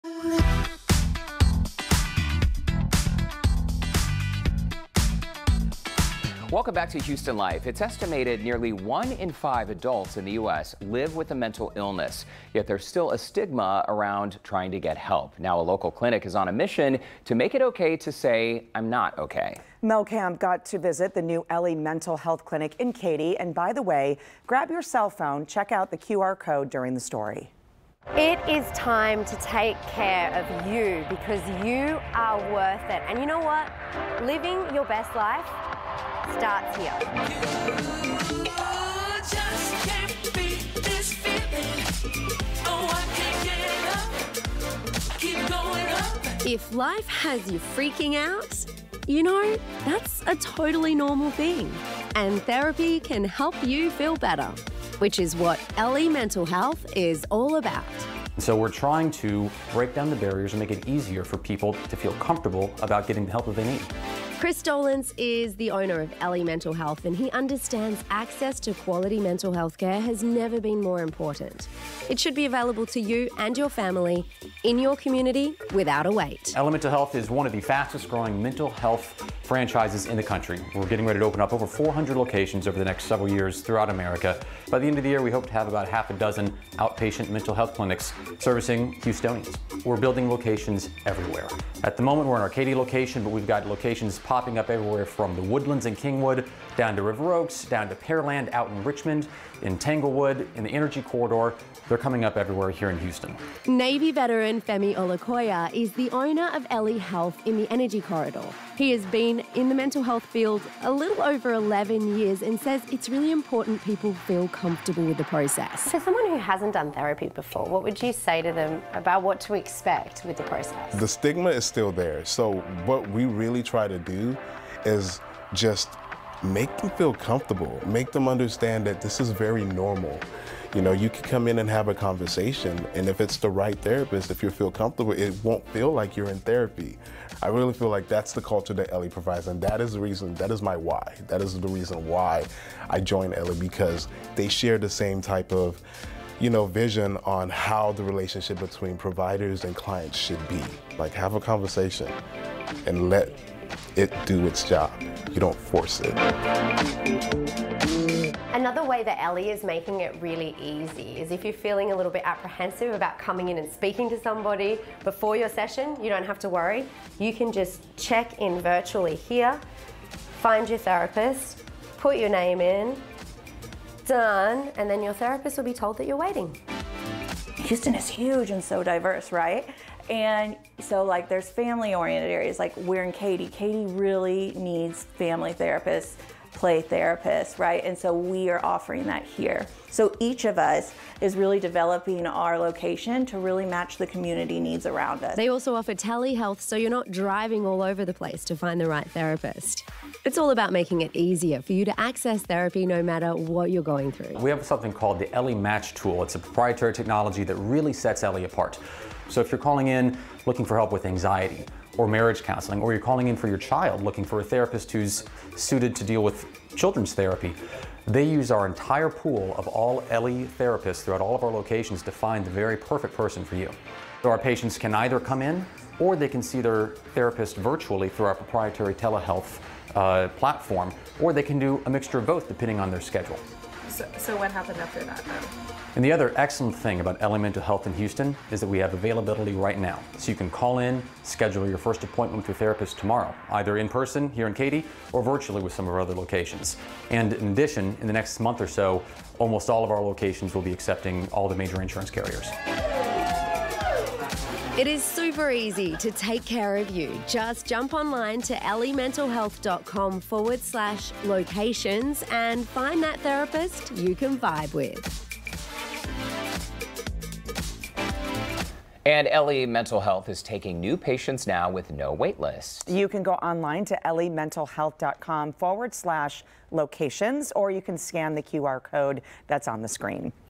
Life Has You Freaking Out? That's Totally Normal and Ellie Mental Health Can Help You Though
If life has you freaking out, you know that’s a totally normal thing right? Therapy can help you feel better. Which is what Ellie Mental health is all about. Ellie Mental Health is opening locations all throughout Houston. It is all about making it easier for you to access therapy. No matter what you’re going through.